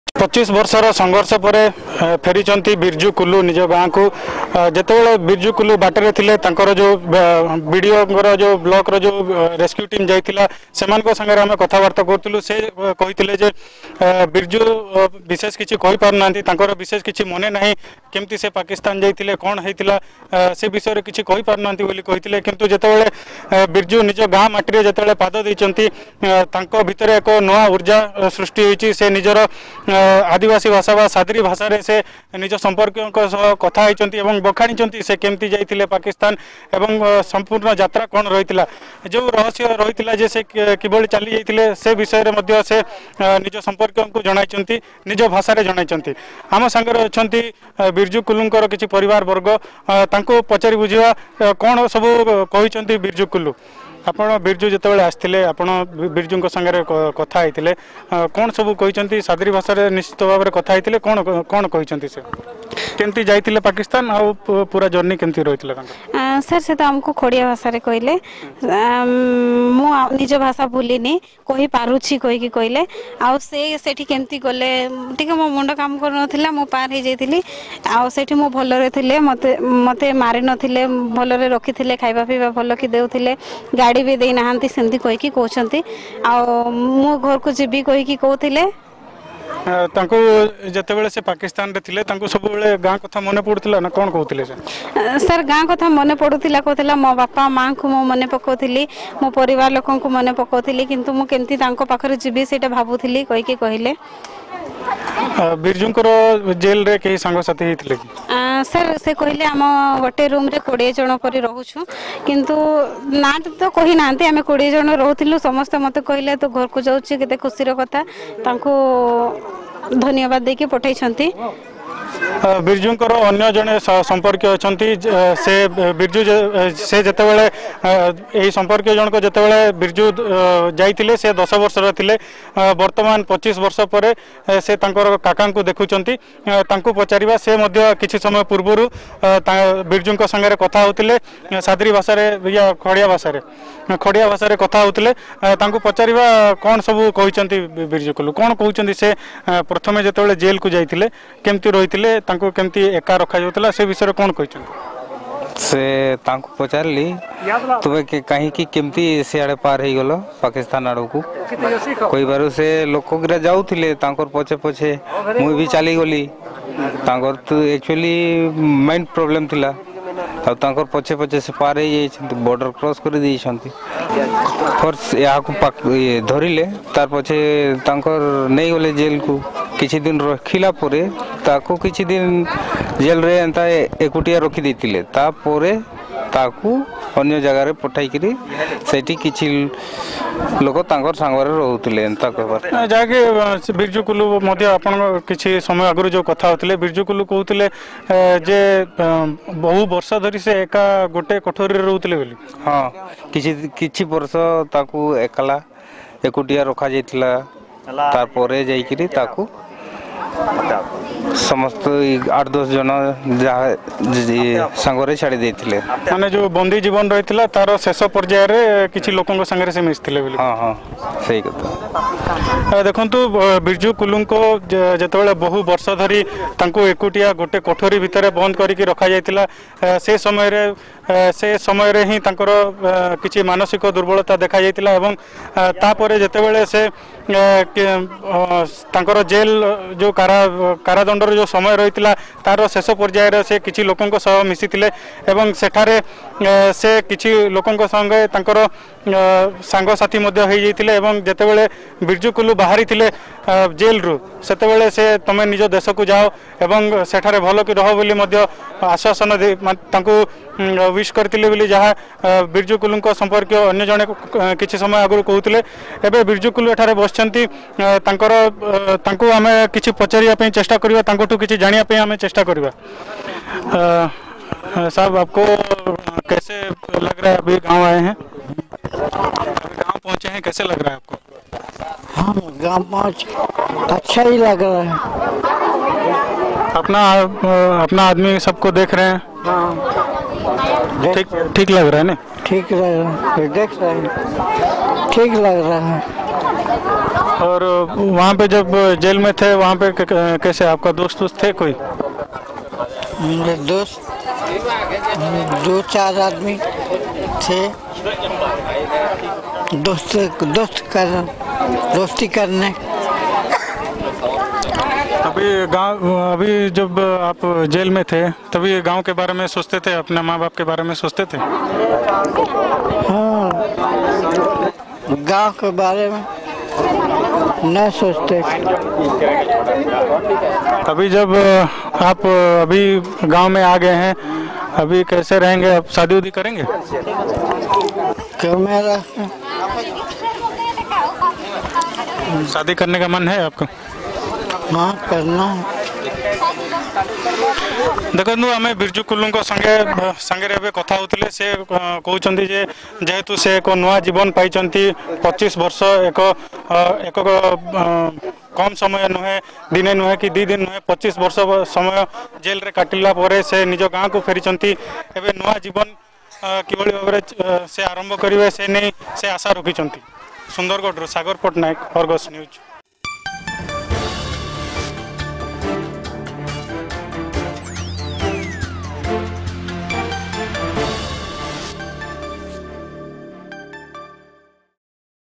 1311 BIRJU BACK DFD2 FB
ଜନ୍ମମାଟିକୁ ଫେରିଛନ୍ତି ବିରଜୁ କୁଲୁ । ପାକିସ୍ତାନ ଲାହୋର ଜେଲରେ 20 ବର୍ଷ କାଟିବା ପରେ ଆଜି ସୁନ୍ଦରଗଡ଼କୁ ଫେରିଛନ୍ତି ବିରଜୁ ...
ଅଧିକ ଖବର ପାଇଁ ଡ଼ାଉନଲୋଡ କରନ୍ତୁ “Argus News” ଆପ୍